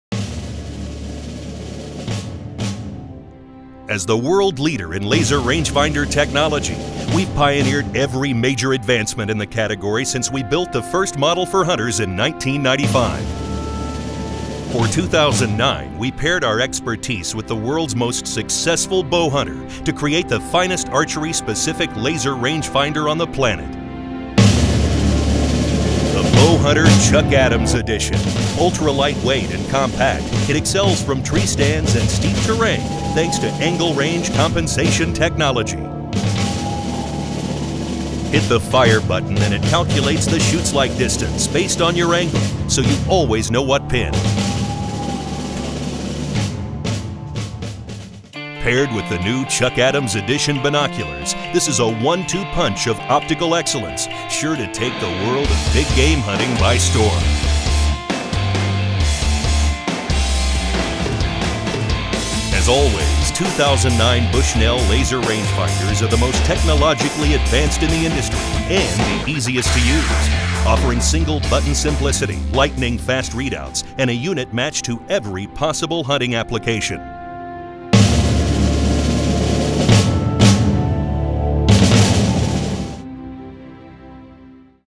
Bushnell - Chuck Adams - Laser Range Finder
A video depicting the new Bowhunter Chuck Adams Edition.

 This video was intended to play on loop.

Demo Reel